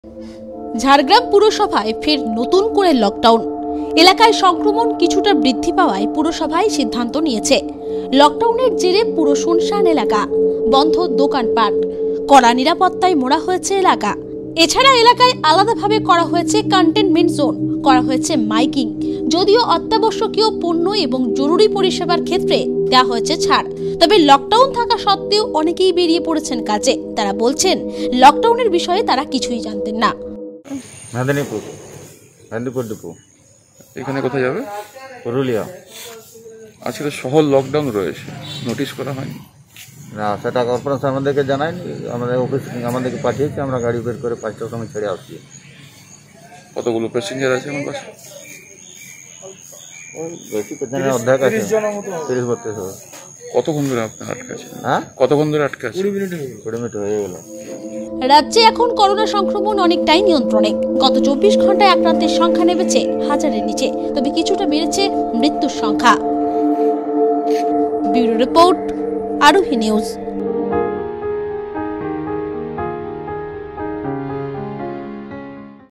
ঝাড়গ্রামে জারি করা হল লকডাউন, ছাড় দেওয়া হয়েছে জরুরি পরিষেবায়
রাজ্যের অন্যান্য জেলায় বিধি নিষেধ শিথিল হলেও ঝাড়গ্রামে নতুন করা জারি করা হয়েছে লকডাউন। এই জেলায় সংক্রমণ কিছুটা বাড়ায় পুরসভার সিদ্ধান্তেই সেখানে মানা হচ্ছে লকডাউন।

@Aarohi News